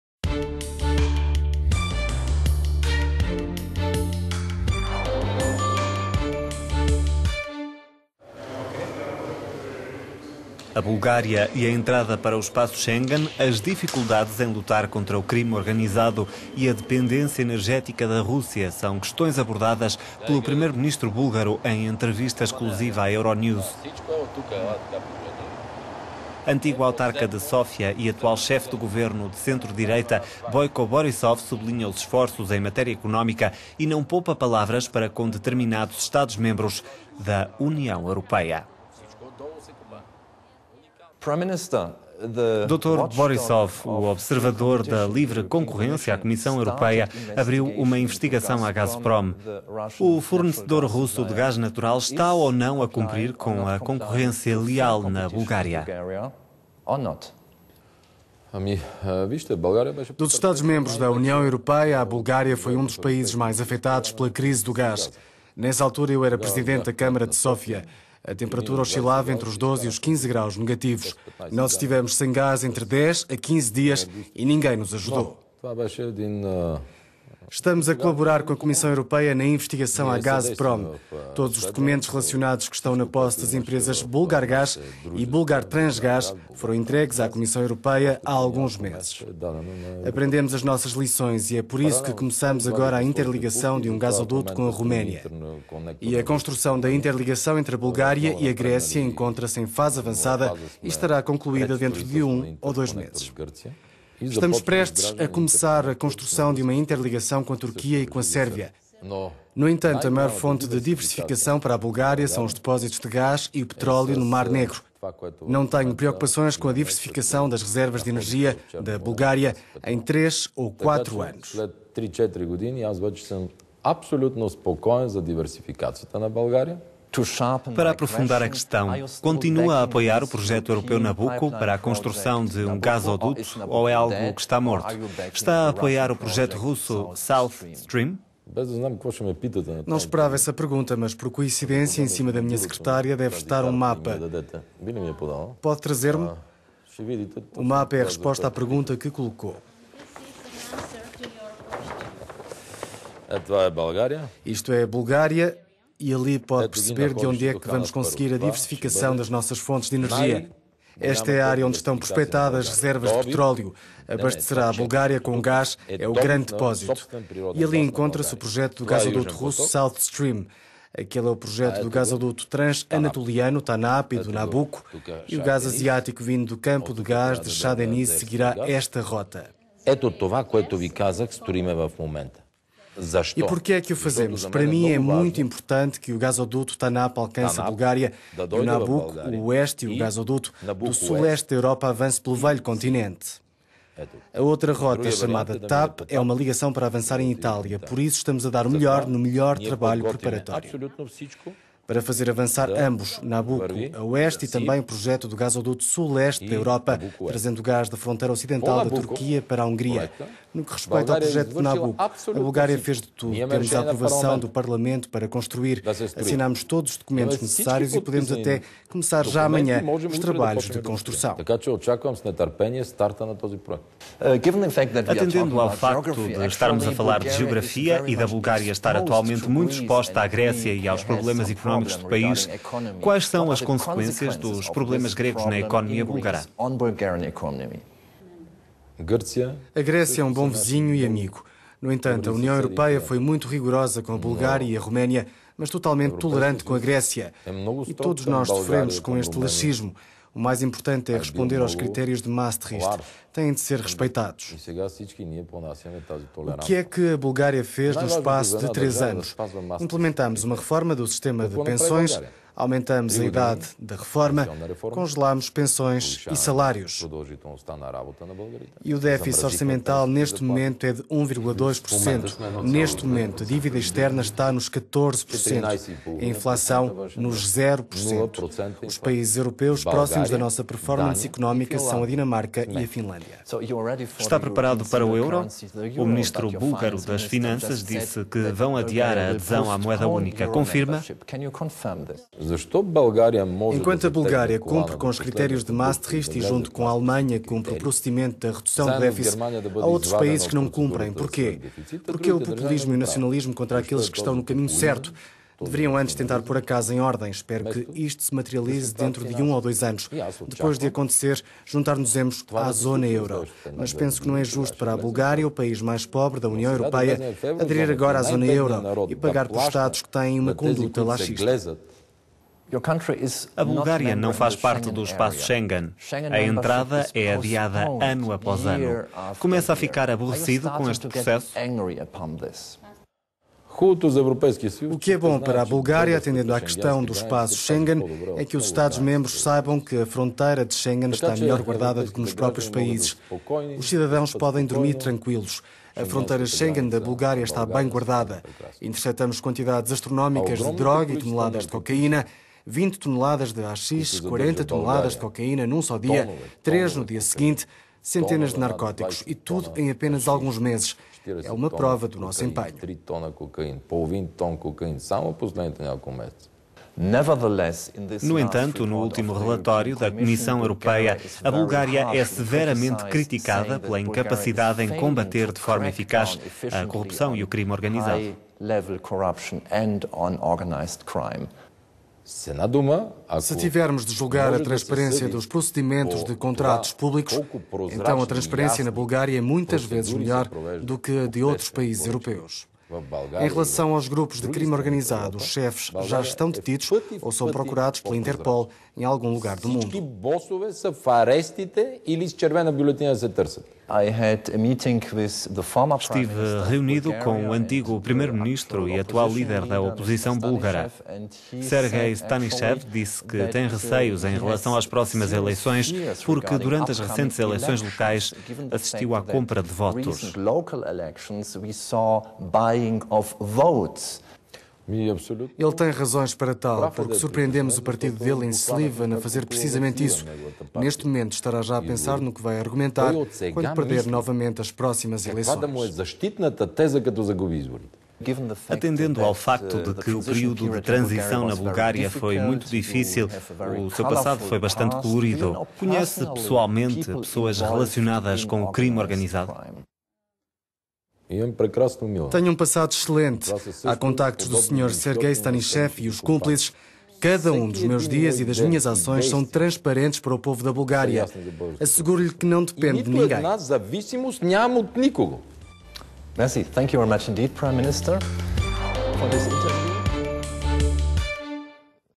euronews interview - Boyco Borissov: "Cada um dos meus dias e ações são transparentes para o povo búlgaro"
A Bulgária e a entrada para o espaço Schengen. As dificuldades em lutar contra o crime organizado e a dependência energética da Rússia são questões abordadas pelo primeiro-ministro búlgaro em entrevista exclusiva à Euronews.

Antigo autarca de Sofia e atual chefe do Governo de centro-direita, Boyko Borrisov sublinha os esforços em matéria económica e não poupa palavras em relação a determinados Estados-membros da União Europeia.

Hans von der Brelie, Euronews: Dr. Borrisov, o observador da livre concorrência, a Comissão Europeia, abriu uma investigação à Gazprom. O fornecedor russo de gás natural está ou não a cumprir com a concorrência leal na Bulgária?

Boyko Borrisov, Primeiro-ministro búlgaro: "Dos Estados-membros da União Europeia, a Bulgária foi um dos países mais afetados pela crise do gás. Nessa altura era Presidente da Câmara de Sofia. A temperatura oscilava entre os 12 e os 15 graus negativos. Nós estivemos sem gás entre dez a 15 dias e ninguém nos ajudou.

Estamos a colaborar com a Comissão Europeia na investigação à Gazprom. Todos os documentos relacionados que estão na posse das empresas Bulgargas e Bulgartransgas, foram entregues à Comissão Europeia há alguns meses.

Aprendemos as nossas lições e é por isso que começámos agora a interligação de um gasoduto com a Roménia. A construção da interligação entre a Bulgária e a Grécia encontra-se em fase avançada e estará concluída dentro de um ou dois meses.

Estamos prestes a começar a construção de uma interligação com a Turquia e com a Sérvia. No entanto, a maior fonte de diversificação para a Bulgária são os depósitos de gás e petróleo no Mar Negro. Não tenho preocupações com a diversificação das reservas de energia da Bulgária em três ou quatro anos."

Hans von der Brelie, Euronews: Continua a apoiar o projeto europeu Nabucco, para a construção de um gasoduto, ou é algo que está morto? Está a apoiar o projeto russo South Stream?"

Boyko Borrisov, Primeiro-ministro búlgaro: "Não esperava essa pergunta, mas por coincidência em cima da minha secretária deve estar um mapa. O mapa é a resposta à pergunta que colocou. Isto é a Bulgária. Ali pode-se perceber de onde é que vamos conseguir a diversificação das nossas fontes de energia. Esta é a área onde estão prospetadas reservas de petróleo. Abastecerá a Bulgária com gás. É o grande depósito. Ali encontra-se o projeto do gasoduto russo South Stream. Aquele é o projeto do gasoduto transanatoliano Tanap e do Nabucco. E o gás asiático vindo do campo de gás de Shah Deniz seguirá esta rota.

E porque é que o fazemos? Para mim é muito importante que o gasoduto Tanap alcance a Bulgária e o Nabucco o oeste e o gasoduto do Sul-Leste da Europa avance para o velho Continente.

A outra rota chamada Tap. É uma ligação para avançar em Itália. Por isso, estamos a dar o melhor no melhor trabalho preparatório, para fazer avançar ambos: Nabucco a oeste e também o projeto do gasoduto Sul-Leste da Europa (trazendo gás da fronteira ocidental da Turquia para a Hungria.)

No que respeita ao projeto do Nabucco, a Bulgária fez de tudo. Temos a aprovação do Parlamento para construir. Assinámos todos os documentos necessários e podemos até começar já amanhã os trabalhos de construção. Estou ansioso com o arranque do projeto do gasoduto Nabucco."

Hans von der Brelie, Euronews: Atendendo ao fato de estarmos a falar de geografia e da Bulgária estar atualmente muito exposta à Grécia e aos problemas económicos do país, quais são as consequências dos problemas gregos na economia búlgara?

Boyko Borrisov, Primeiro-ministro búlgaro: "A Grécia é um bom vizinho e amigo. No entanto, a União Europeia foi muito rigorosa com a Bulgária e a Roménia, mas totalmente tolerante com a Grécia. E todos nós sofremos com este laxismo. O mais importante é responder aos critérios de Maastricht. Têm de ser respeitados.

O que é que a Bulgária fez no espaço de três anos? Implementámos uma reforma do sistema de pensões. Aumentámos a idade da reforma. Congelámos pensões e salários. O défice orçamental neste momento é de 1,2%. A dívida externa está nos 14% e a inflação nos 0%.

Os países europeus próximos da nossa performance económica (atendendo aos critérios de Maastricht) são a Dinamarca e a Finlândia."

Hans von der Brelie, Euronews: Está preparado para o euro? O ministro búlgaro das Finanças disse que vão adiar a adesão à moeda única. Confirma?